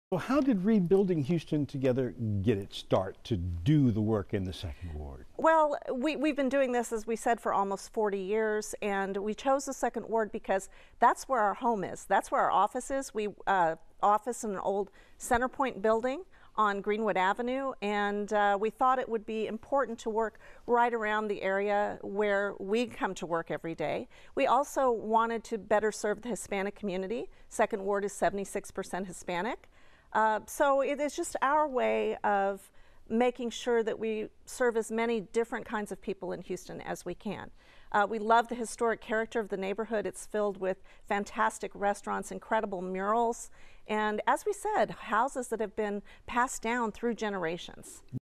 Why the Second Ward?
Executive Director Christine Holland explains to Fox26's Jose Grinan why Rebuilding Together Houston is working in the Second Ward.